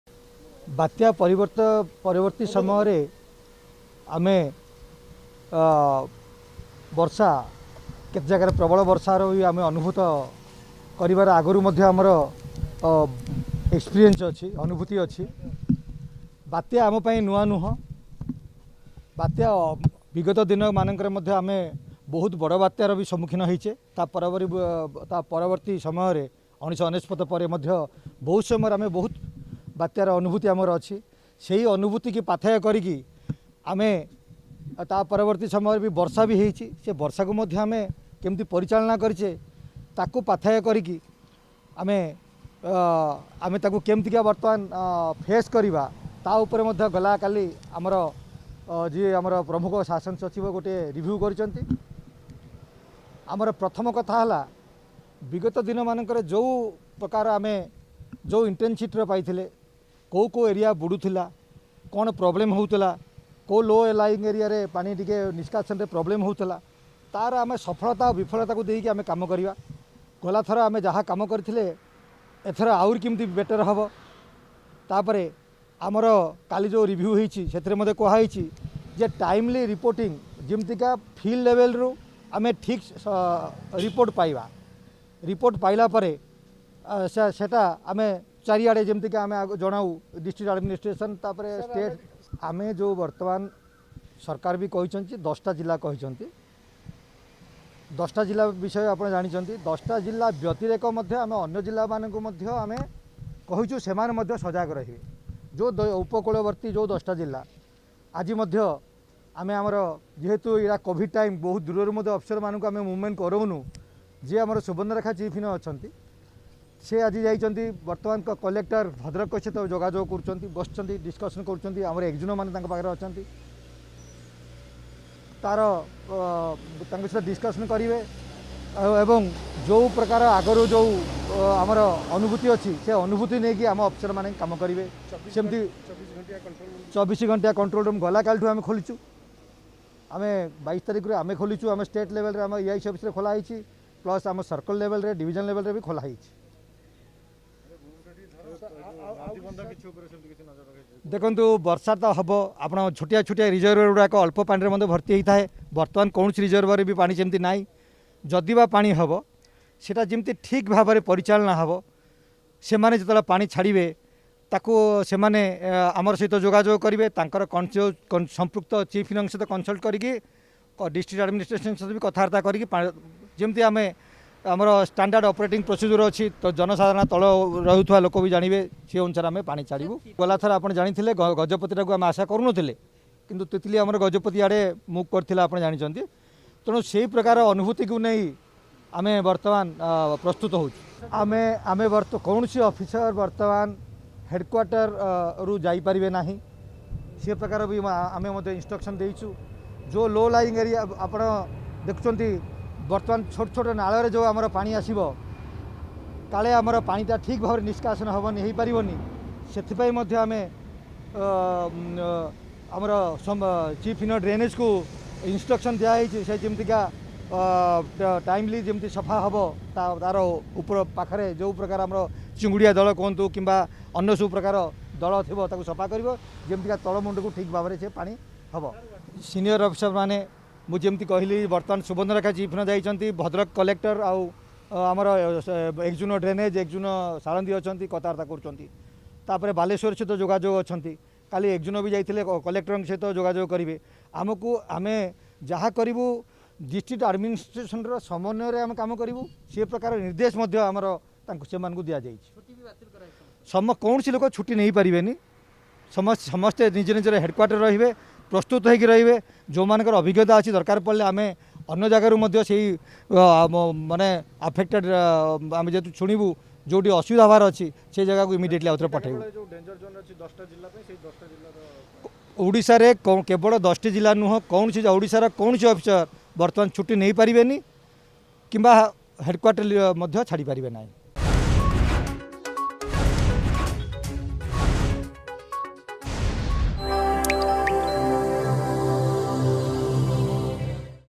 ବାତ୍ୟା ଆମ ପାଇଁ ନୂଆ ନୁହେଁ। ଅନେକ ବାତ୍ୟାର ଅନୁଭୂତି ଅଛି।
ଶୁଣନ୍ତୁ ସମ୍ଭାବ୍ୟ ବାତ୍ୟା ନେଇ କଣ ପ୍ରତିକ୍ରିୟା ରଖିଛନ୍ତି ଜଳ ସମ୍ପଦ ବିଭାଗ ସର୍ବୋଚ୍ଚ ଯନ୍ତ୍ରୀ ଜ୍ୟୋତିର୍ମୟ ରଥ।